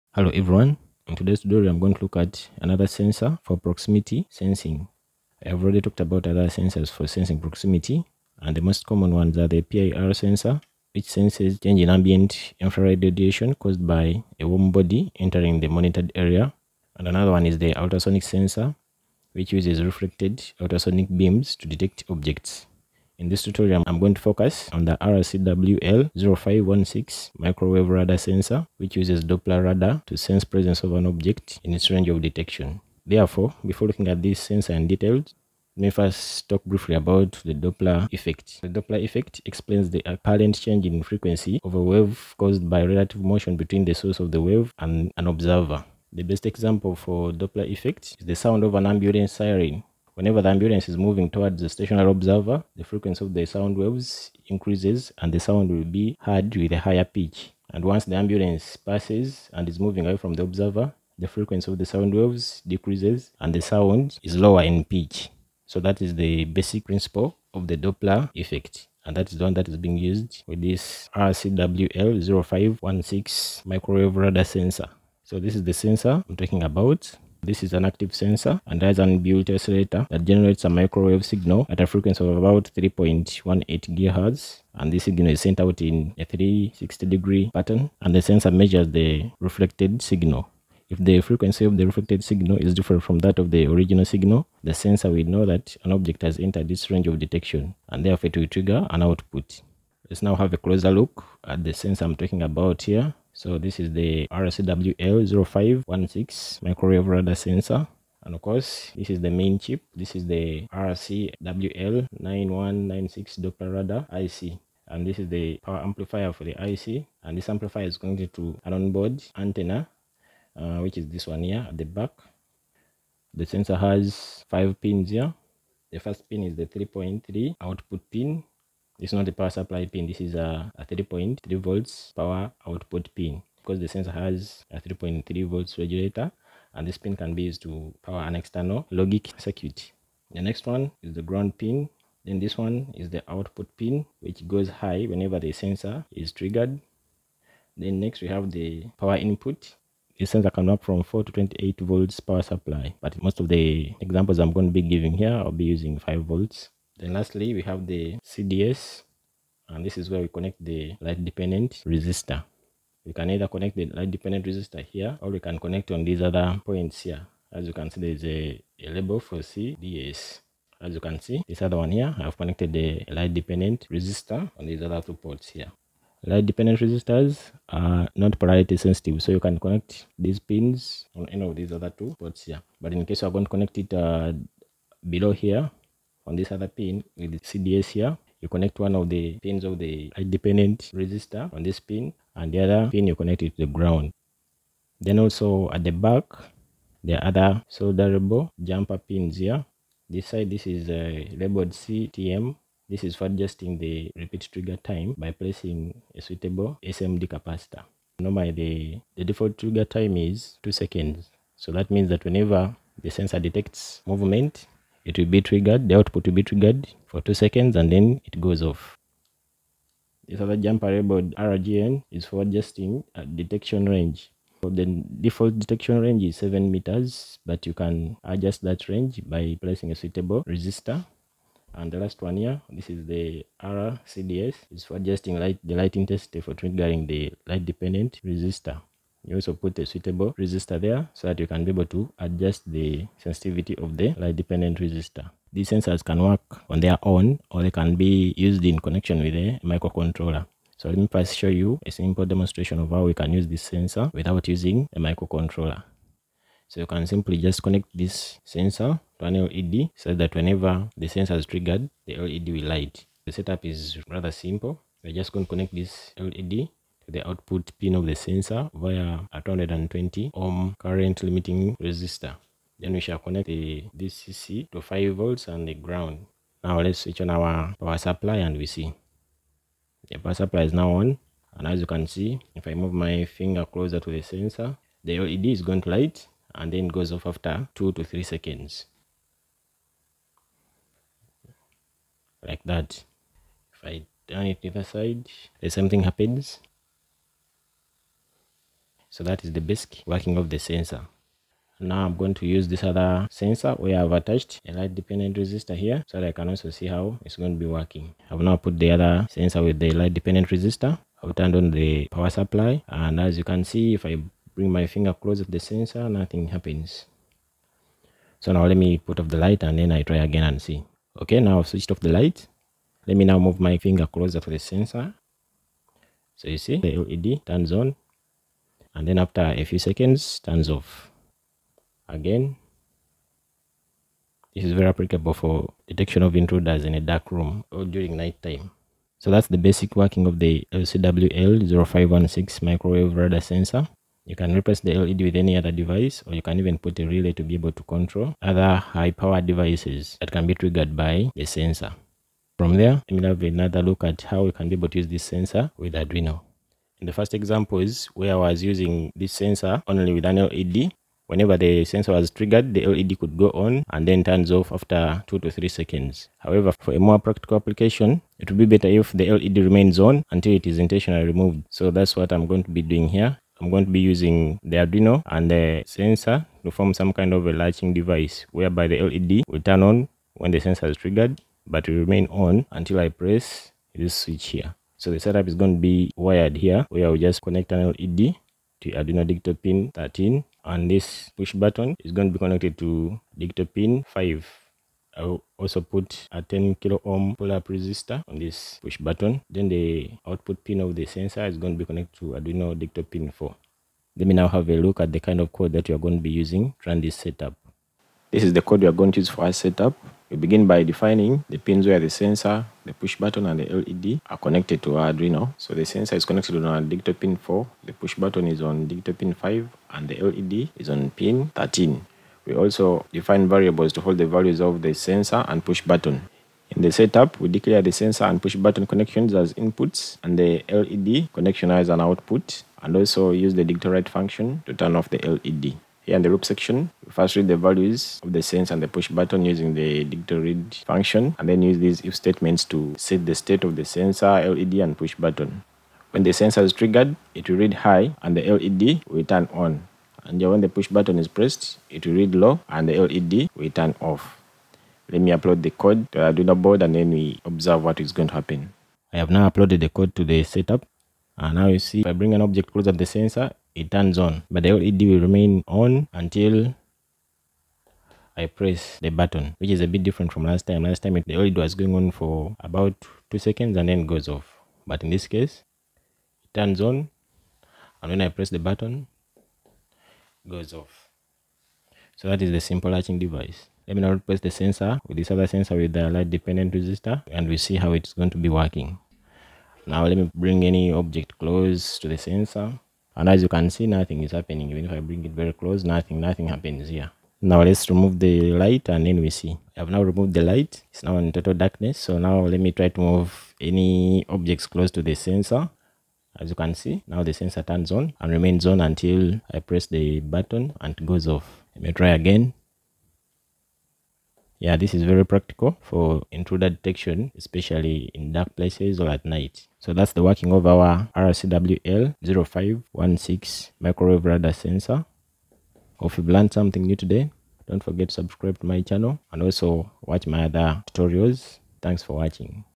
RCWL 0516 Microwave Radar Proximity Sensor | With and Without Arduino
This tutorial is a detailed look at the RCWL-0516 Microwave radar sensor which is a cheap device that uses Doppler radar for proximity detection and can work on its own or with a microcontroller like Arduino.